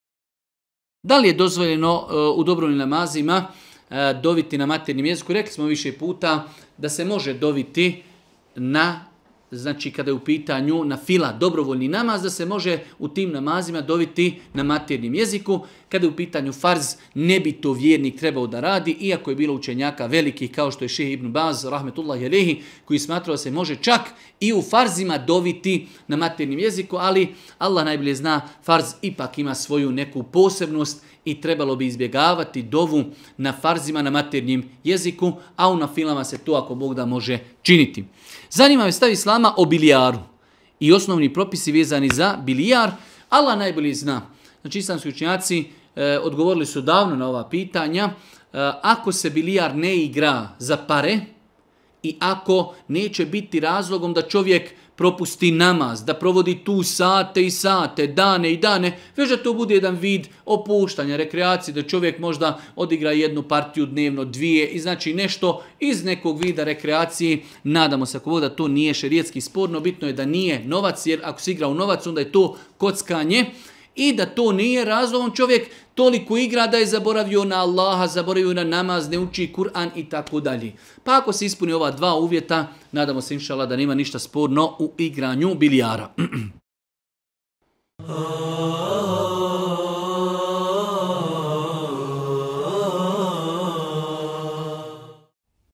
Da li se može doviti na maternjem jeziku? - mr. Elvedin Pezić
Da li možemo doviti na maternjem jeziku u dobrovoljnom namazu? - mr. Elvedin Pezić
● Pretplatite se na naš kanal: